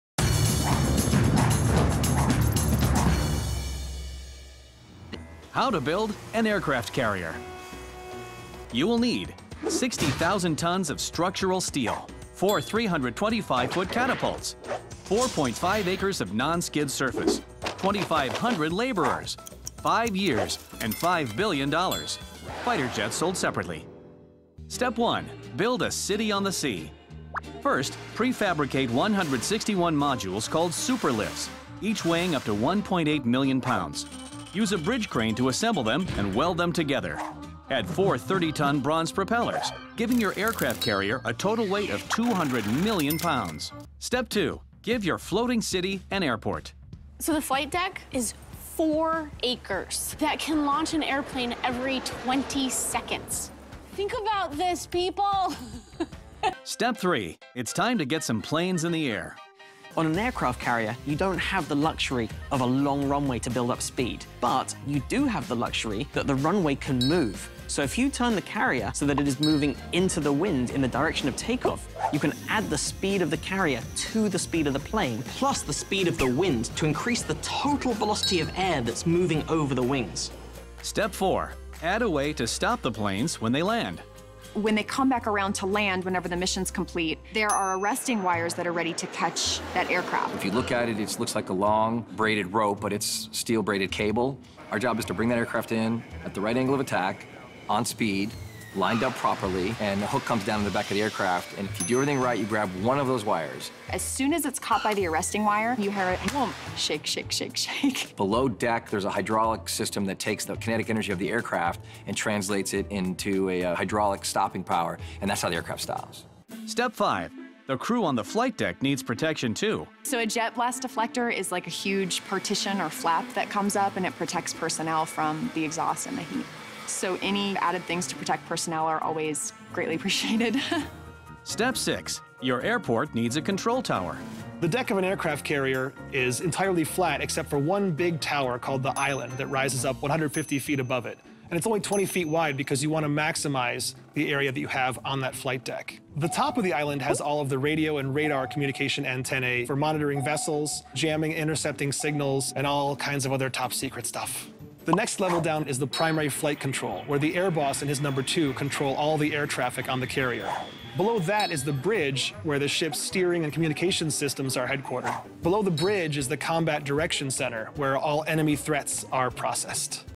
Cómo construir un portaaviones [4K]
Un curioso vídeo enseña los materiales y las técnicas necesarias para construir un buque de guerra capaz de transportar y operar aviones

Para llevar a cabo el audaz proyecto, son necesarios unos ‘pocos' materiales: 60.000 toneladas de acero estructural, 4 catapultas de aviones, 4,5 hectáreas de superficie antideslizante, 2.500 trabajadores, 5 años y unos 5.000 millones de dólares.

El proceso es muy ‘sencillo'. El primer paso es "construir una ciudad sobre el mar" con la ayuda de un puente-grúa. Luego, es necesario agregar un aeropuerto que cuente con un sistema de aterrizaje y una torre de control. Y ¡listo!